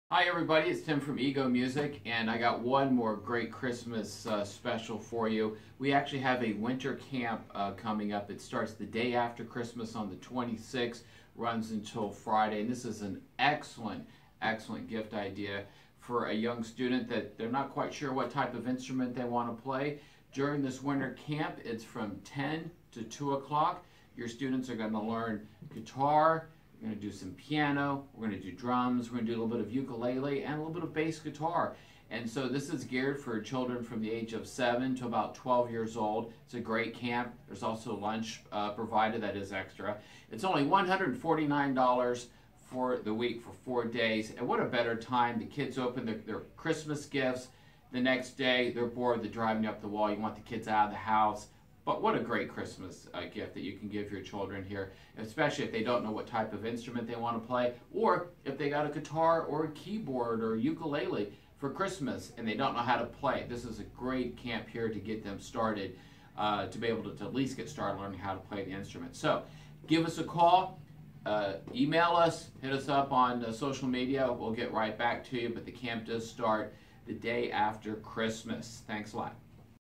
Winter Camp
Camp runs Tuesday 12/26 through Friday 12/30, 10AM to 2PM each day.
Campers will play piano, drums, ukulele, guitar, AND bass guitar every day !
Perfect for ages 7 to 13, beginning to intermediate experience levels.
No instrument needed, we'll have instruments for the kids to use there !